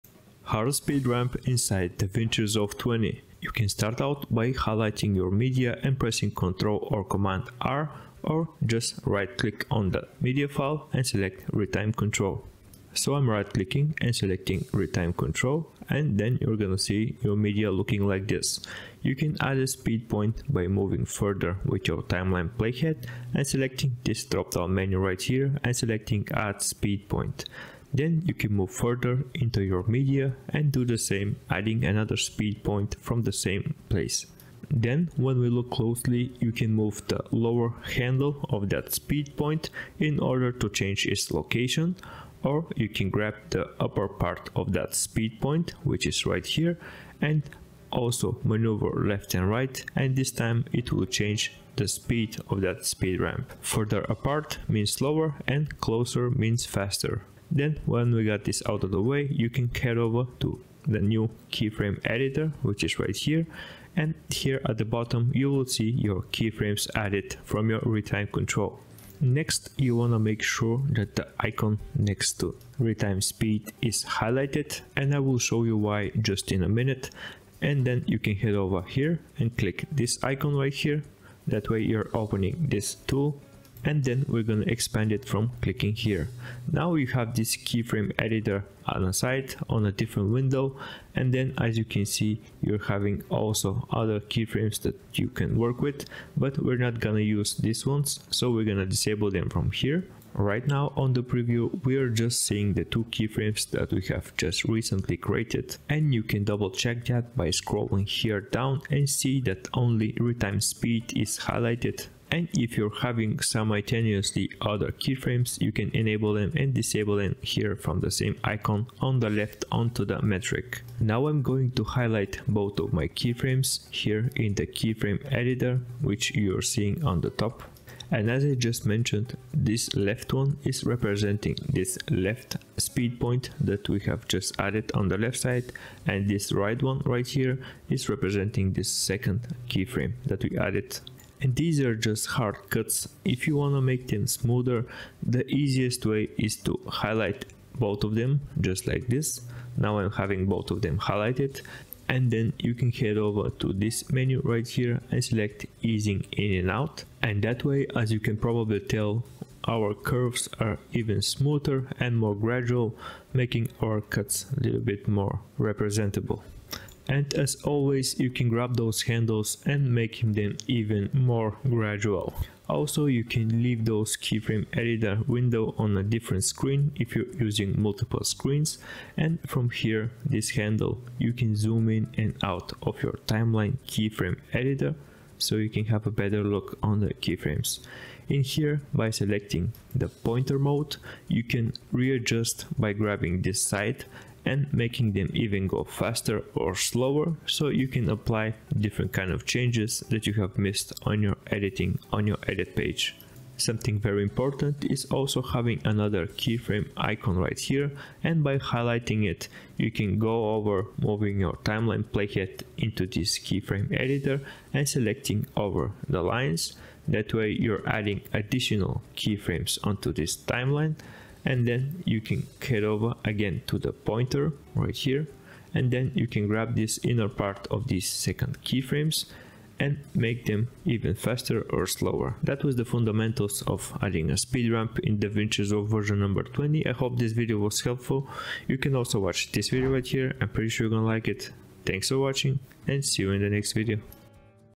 How To SPEED RAMP In Davinci Resolve 20 Tutorial
In This Video I Will Show You How To SPEED RAMP In Davinci Resolve 20 Tutorial